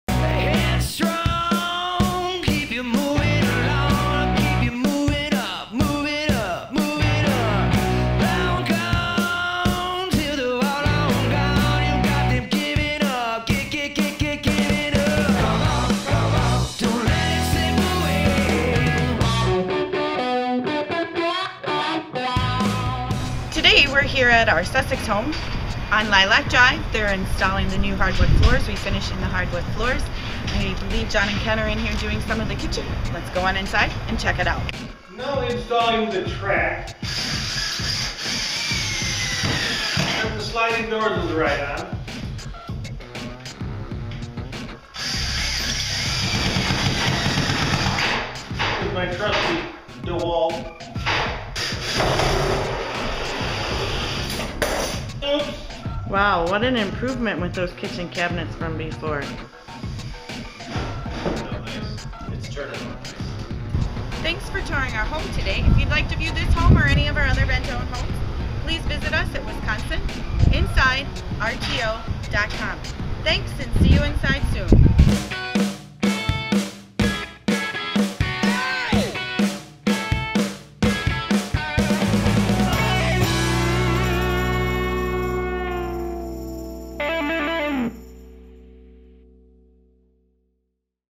Milwaukee Team - Update on Lilac Dr. Project in Sussex, WI
Stacey gives a quick look at the new kitchen additions as well as the closet's sliding doors.